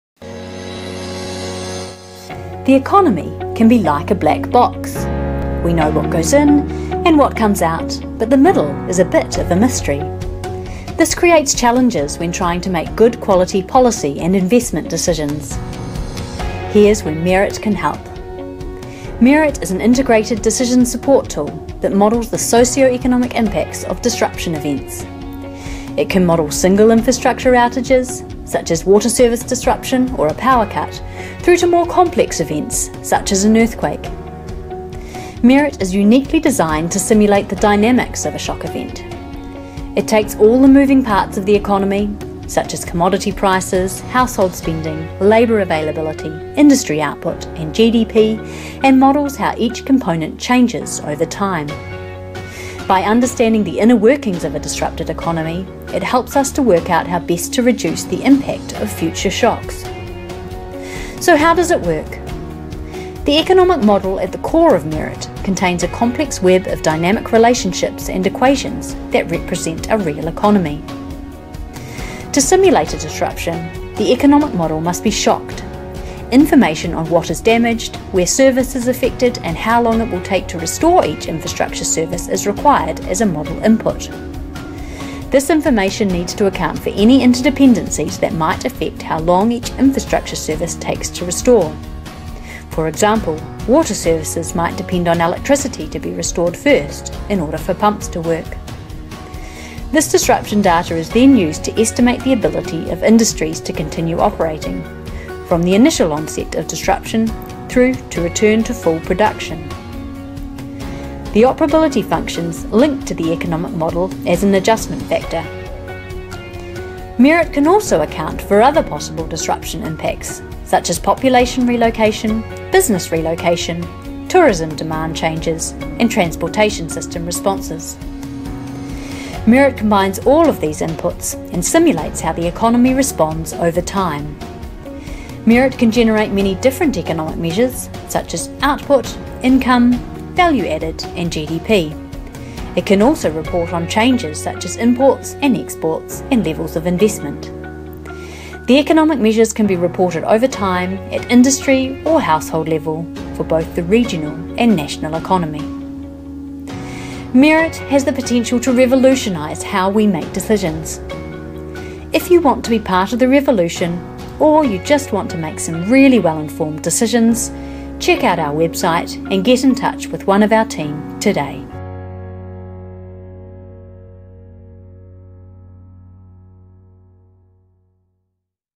What is MERIT
An introduction to the MERIT (Measuring the Economics of Resilient Infrastructure Tool) suite of ‘Integrated Spatial Decision Support Systems’ that estimate the economic consequences associated with disruption events. Disruptions are evaluated through time (i.e. it covers both the event and the subsequent response, recovery and rebuild phases), across space (i.e. by detailed spatial location, region and nation as a whole) and for multiple stakeholders (i.e. households and industries).